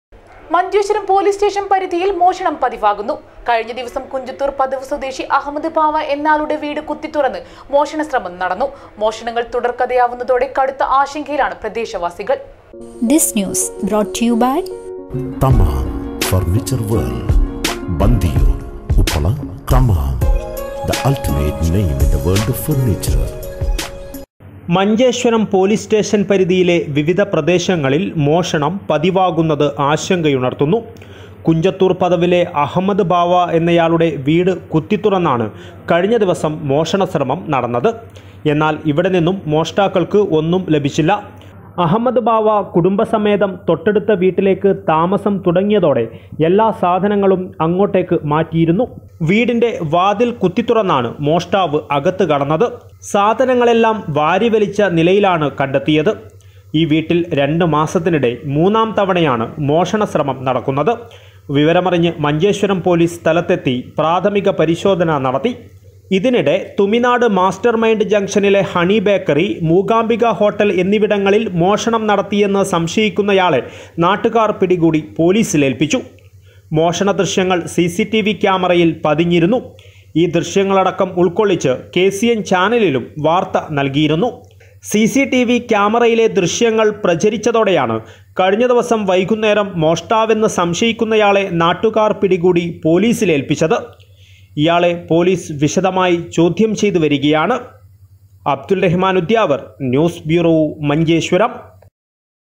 മഞ്ചേശ്വരം പോലീസ് സ്റ്റേഷന്‍ പരിധിയില്‍ മോഷണം പതിവാകുന്നു
മഞ്ചേശ്വരം പോലീസ് സ്റ്റേഷന്‍ പരിധിയില്‍ മോഷണം പതിവാകുന്നു. കഴിഞ്ഞ ദിവസം കുഞ്ചത്തൂര്‍ പദവ് സ്വദേശി അഹമ്മദ് ബാവാ എന്നയാളുടെ വീട് കുത്തിത്തുറന്ന് മോഷണ ശ്രമം നടന്നു. മോഷണങ്ങള്‍ തുടര്‍ക്കഥയാകുന്നതോടെ കടുത്ത ആശങ്കയിലാണ് പ്രദേശ വാസികള്‍.